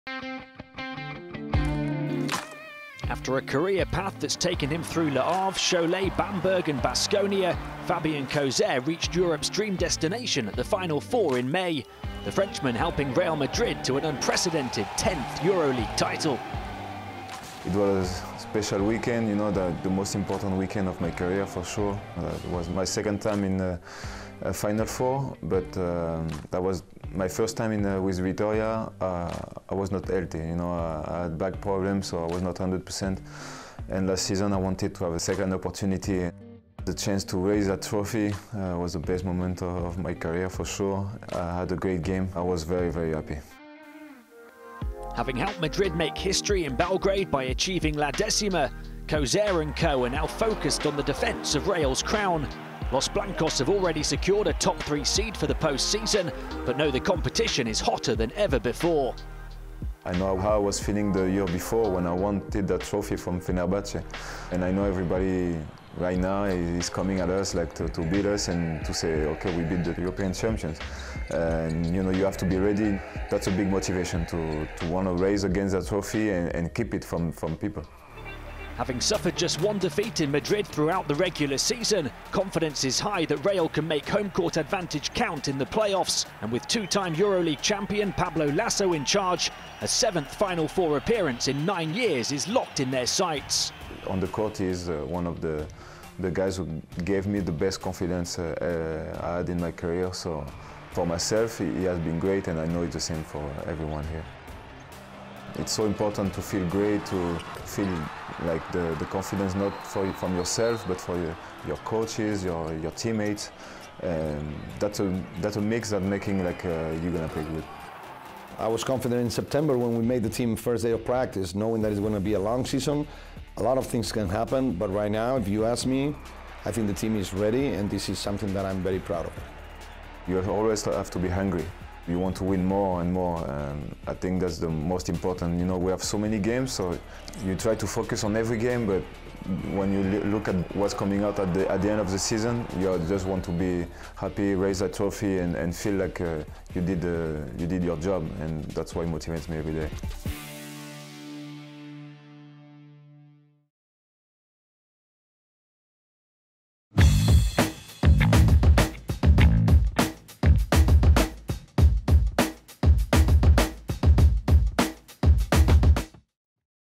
Focus on: Fabien Causeur, Real Madrid
After he was defending champion Real Madrid's top scorer in the Turkish Airlines EuroLeague Championship Game last spring in Belgrade, guard Fabien Causeur is out to relive those good vibrations again.